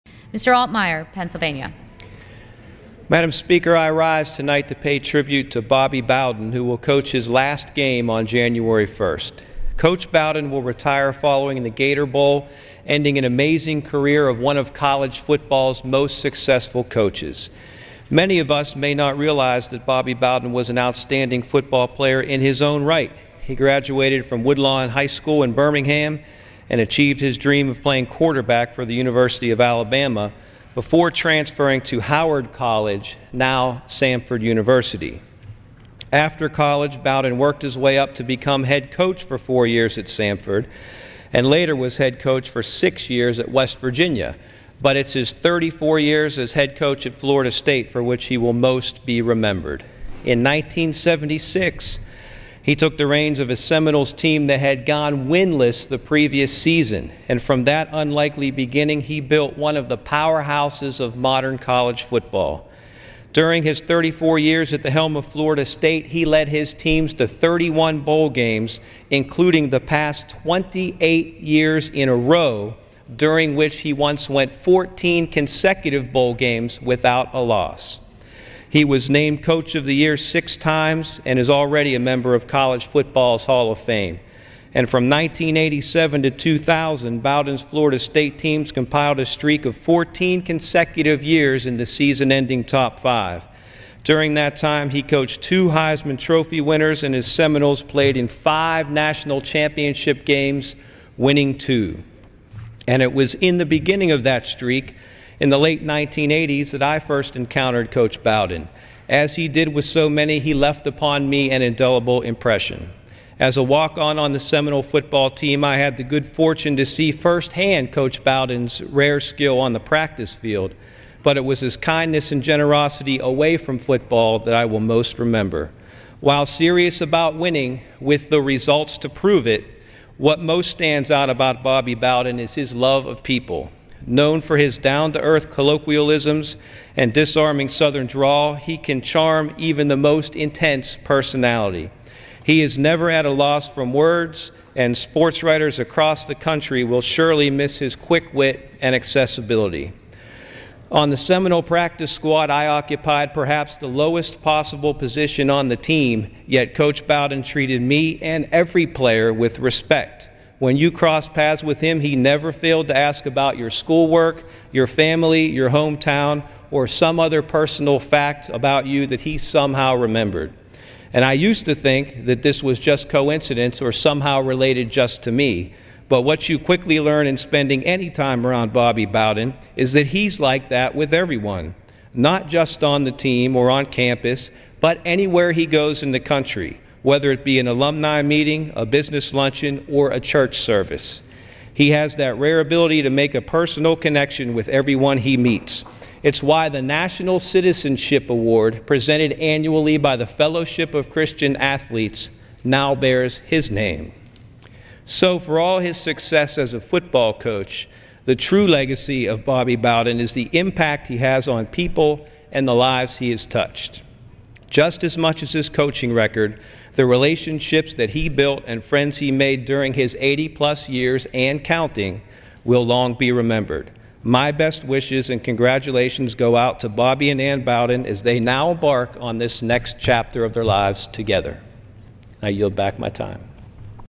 Tribute to Bobby Bowden
Congressman Jason Altmire pays tribute to legendary college football coach Bobby Bowden upon Bowden's retirement from Florida State in 2009.